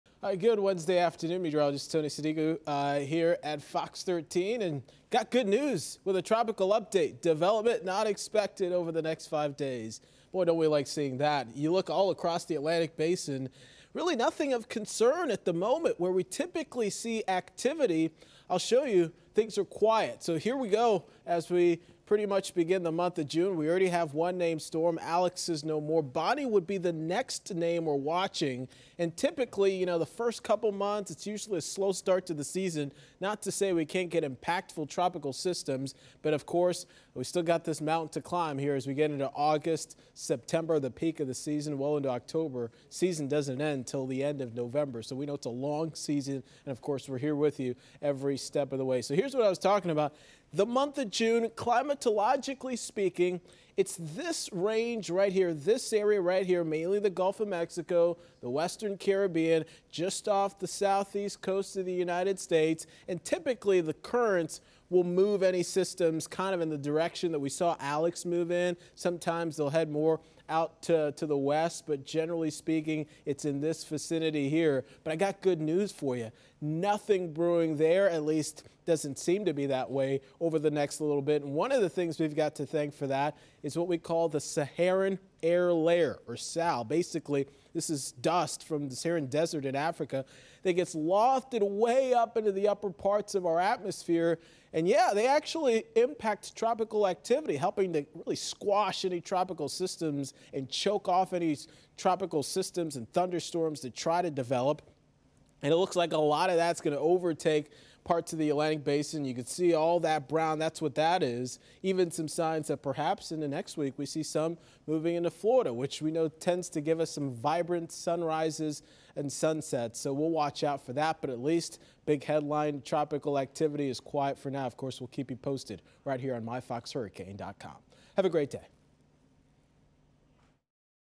Tropics forecast: June 8, 2022 - Atlantic Hurricane Season
Meteorologist Tony Sadiku shows us the outlook for potential tropical weather after the first week of June brought the first named storm of the 2022 Atlantic Hurricane Season.